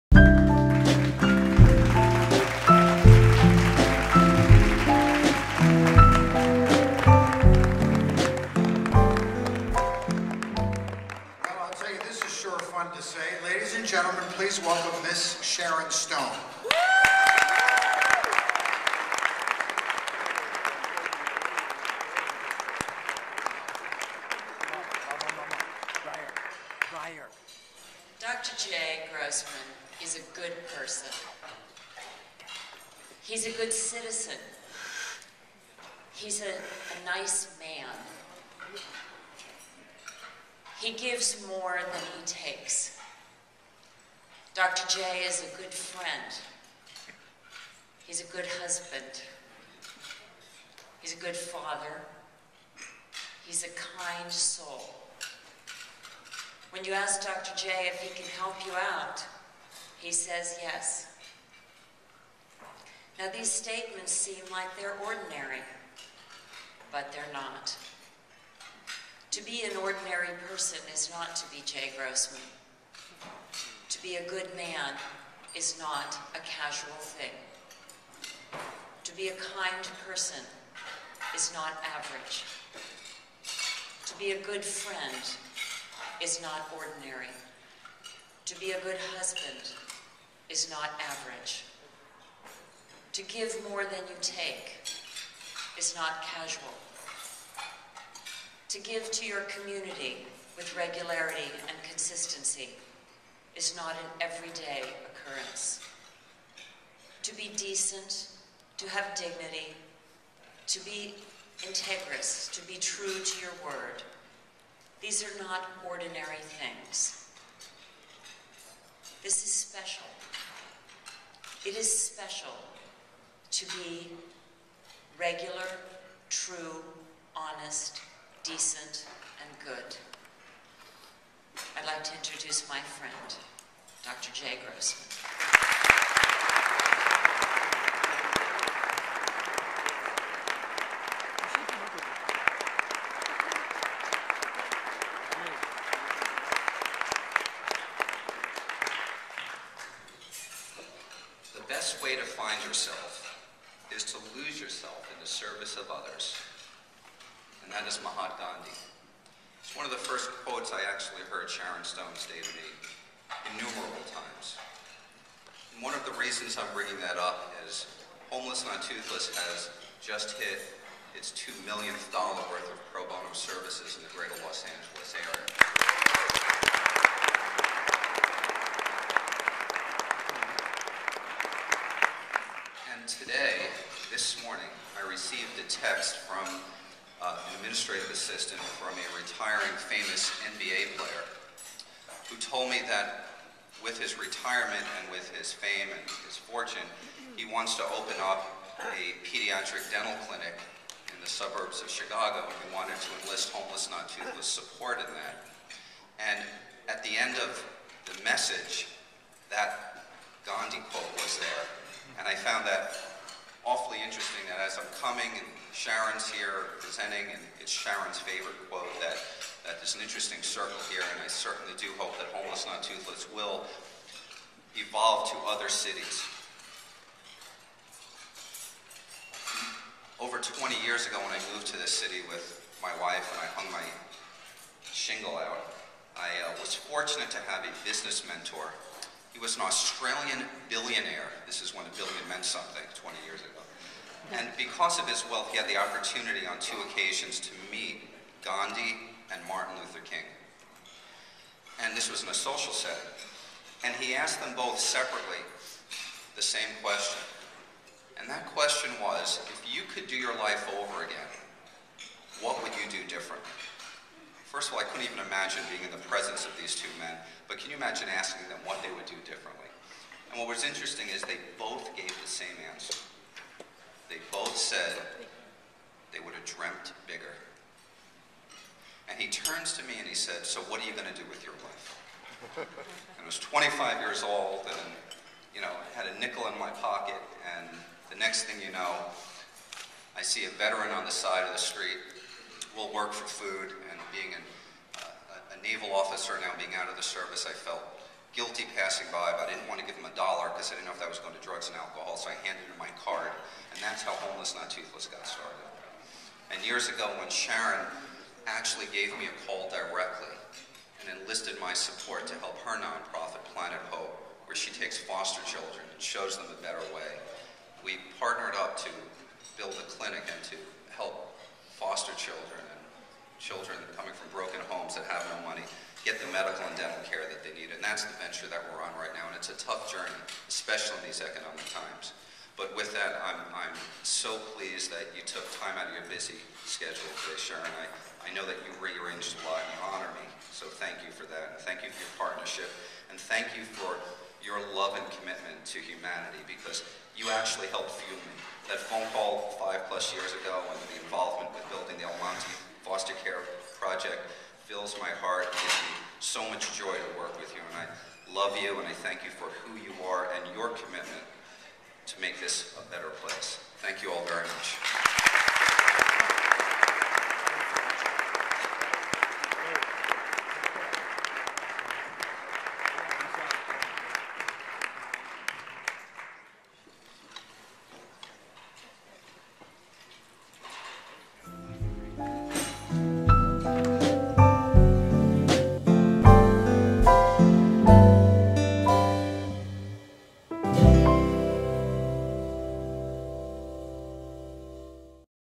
Stone Vic award DrJ.mov
Sharon Stone presenting a philanthropic award to Dr Jay Grossman, founder of Homeless Not Toothless at the VIC (Valley Interfaith Council) Gala May 2011